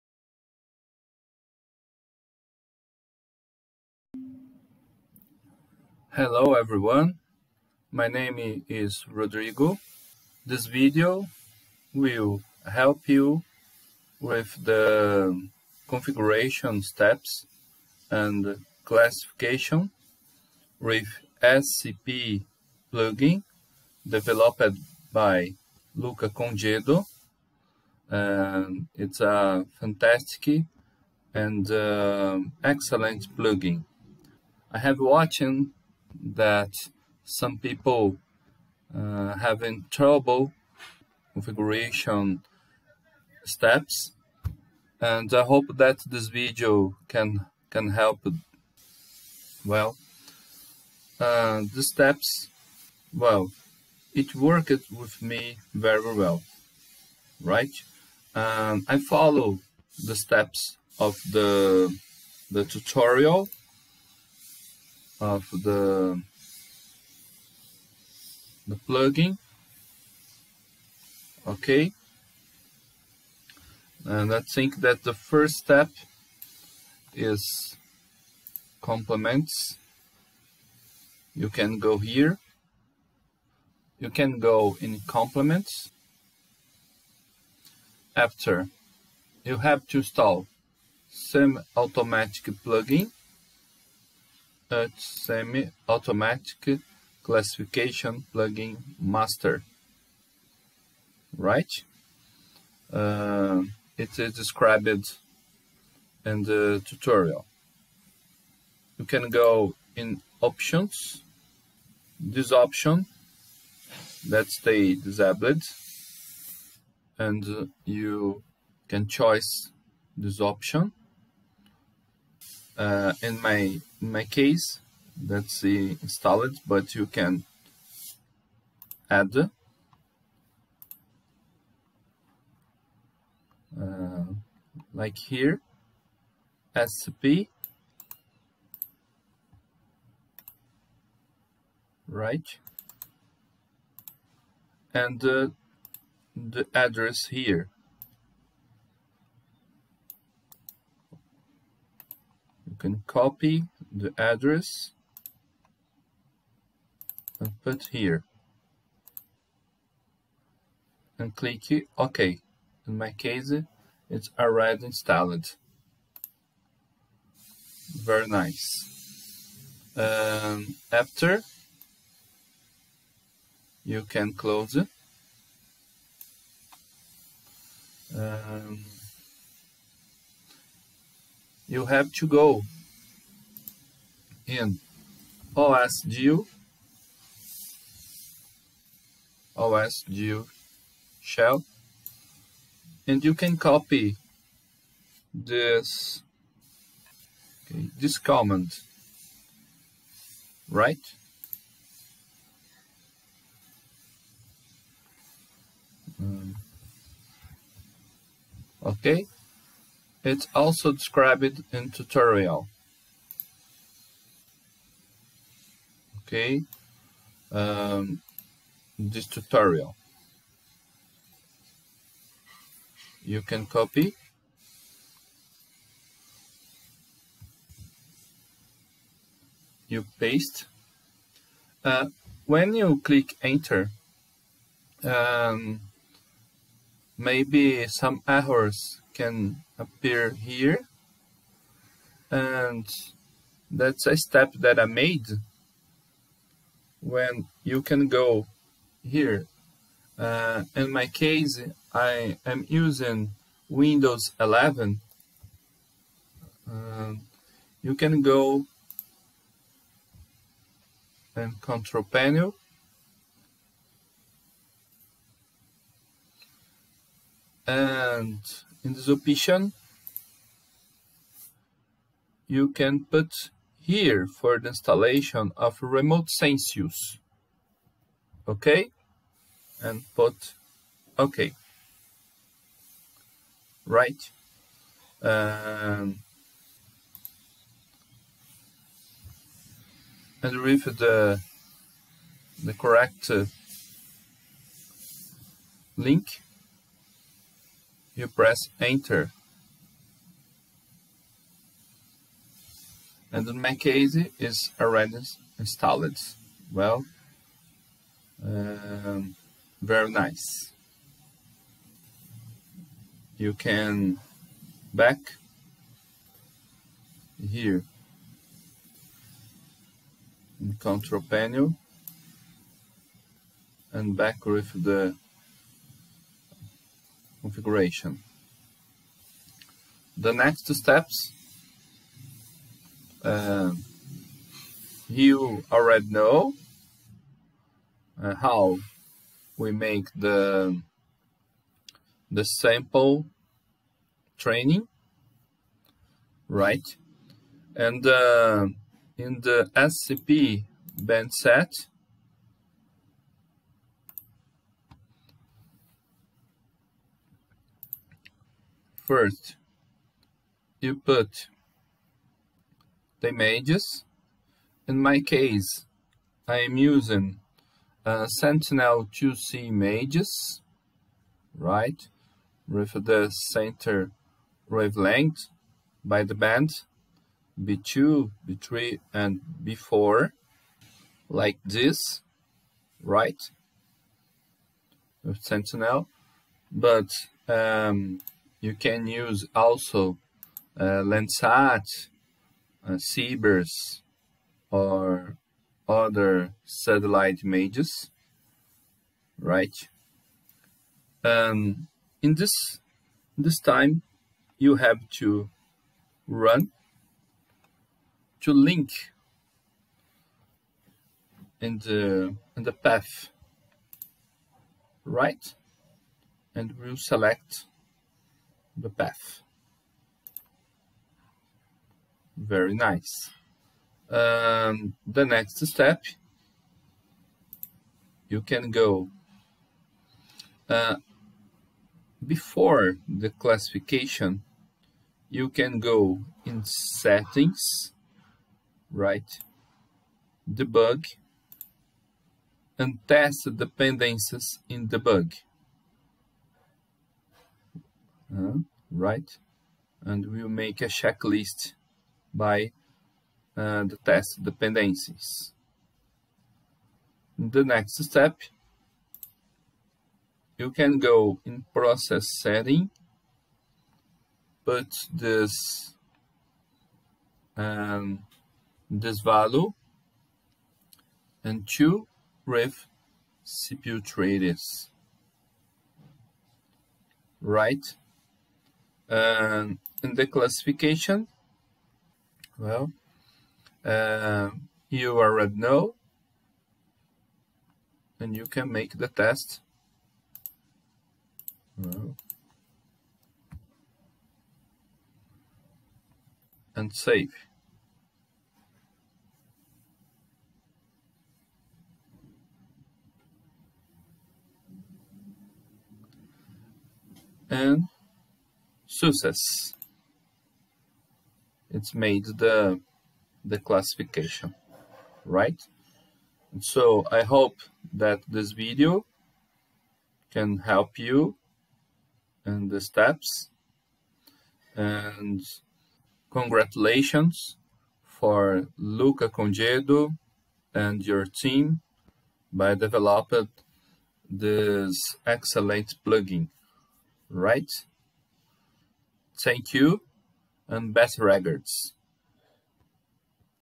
SCP and Remotior Sensius Plugin for image classification
This video shows some steps for instalation, configuration and image processing with SCP and Remotior Sensius plugin that worked well for me.
I hope that this video can help some people with problem in this steps
Congratulations for Luca Congedo and your team for the excelent and didatic plugin!
Best Regards!
Rodrigo